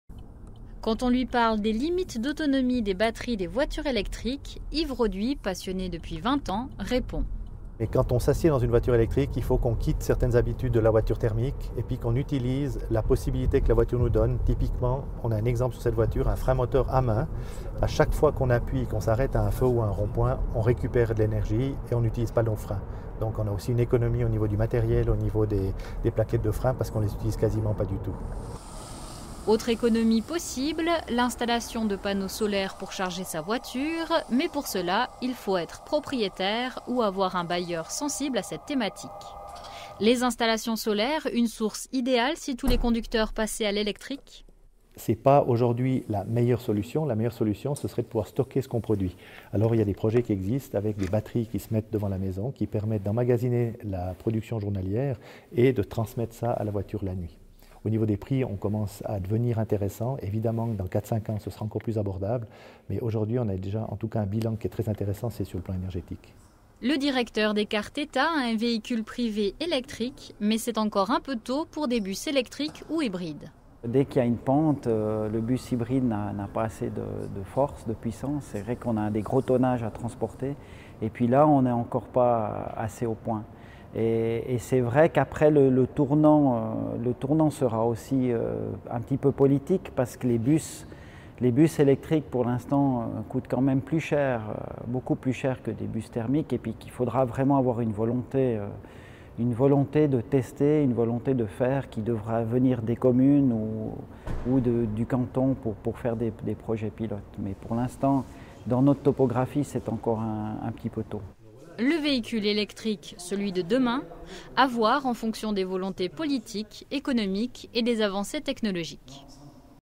Plutôt que le véhicule de demain, l’électrique sera peut-être celui d’après-demain
3525 véhicules électriques de plus en Suisse en 2016. Cela représente 1,1% de toutes les nouvelles mises en circulation de voitures de tourisme pour cette année-là. L'électrique n'est pas le carburant le plus coté en Suisse. Yves Roduit, ingénieur électricien, en est pourtant convaincu: c'est l'avenir. Tour d'horizon des limites et avantages de ce type de transport.